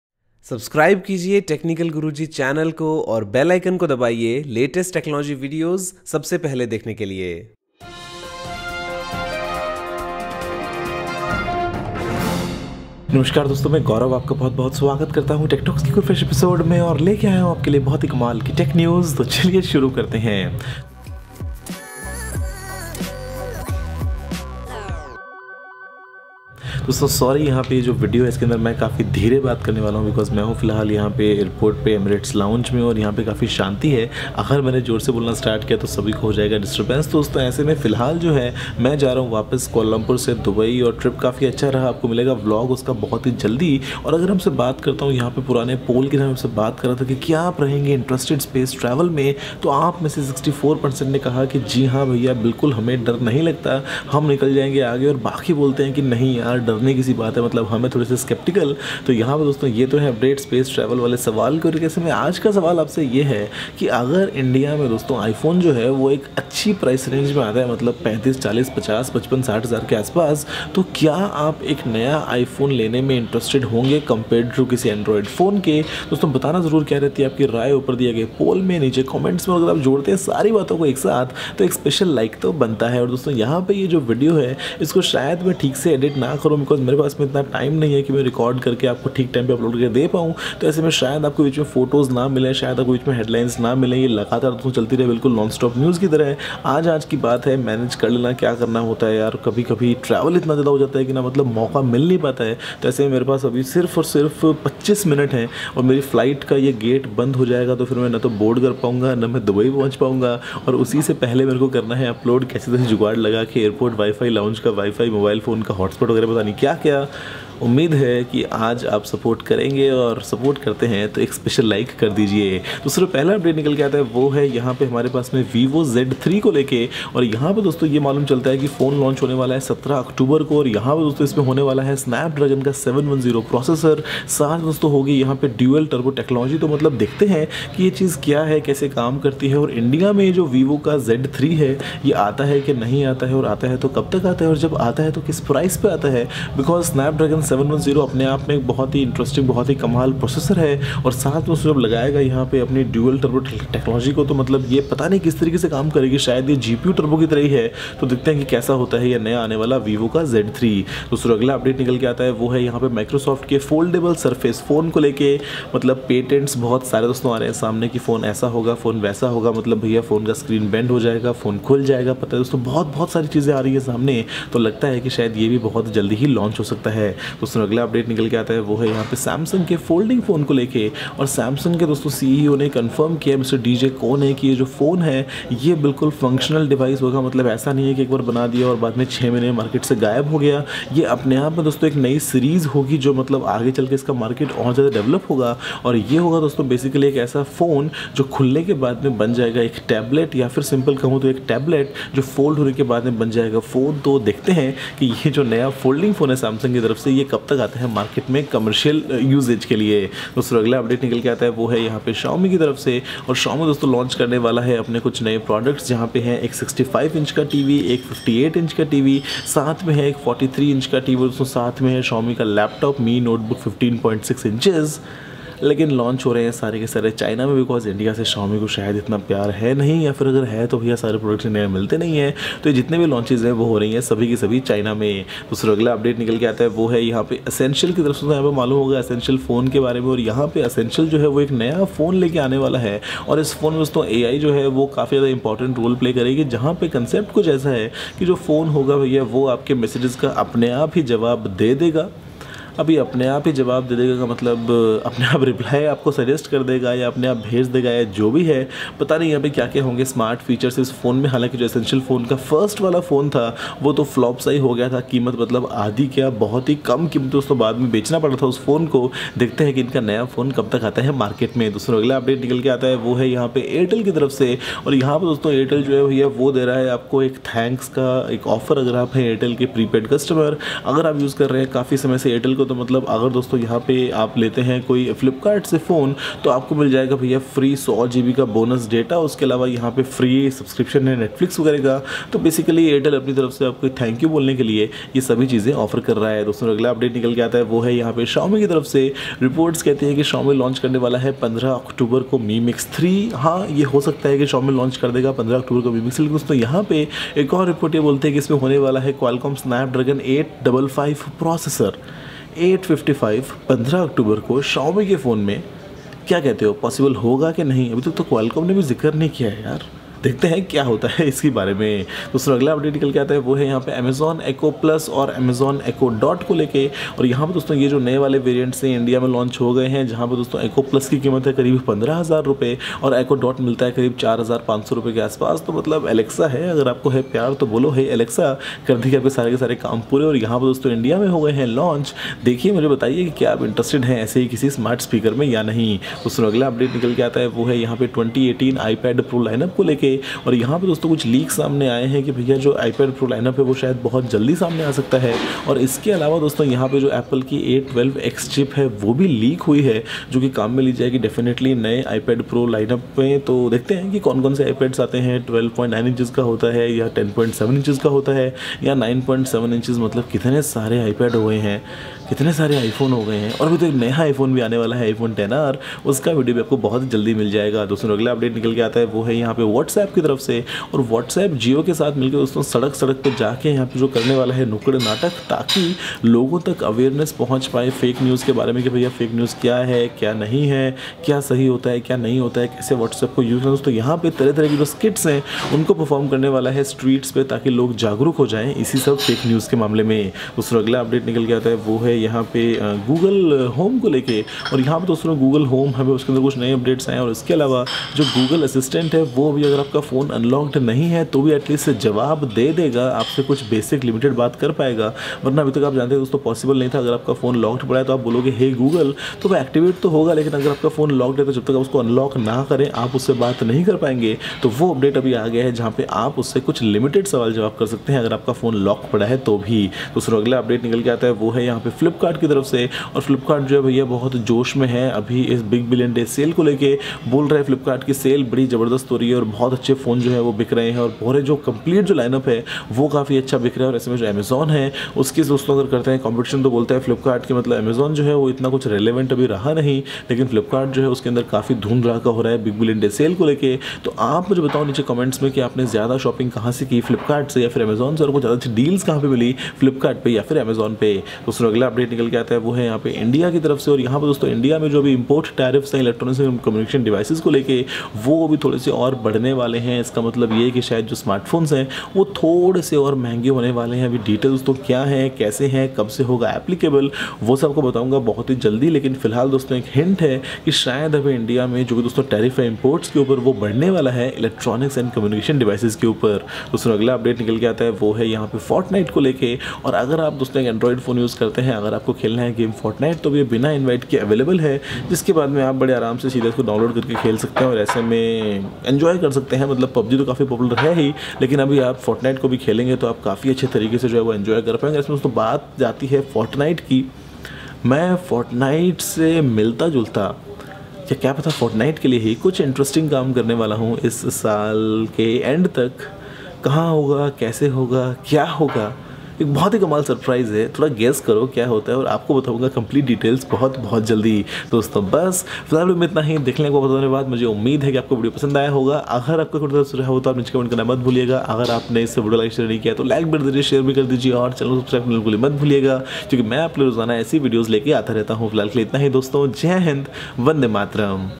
Tech Talks #632 - Snapdragon 855 Mi Mix 3, Samsung Folding Tablet, Fortnite Android, Flipkart Sale
Namaskaar Dosto, Tech Talks ke is Episode mein maine aapse kuch interesting Tech News Share ki hai jaise Vivo Z3 Launch, Samsung Folding Phone Launch,  Microsoft Surface Phone, Fortnite on Android, Essential Phone next version, 2018 iPad Pro Lineup, Xiaomi TV Launch, Google Assistant aur bahut kuch. Mujhe umeed hai ki yeh video aapko pasand aayega.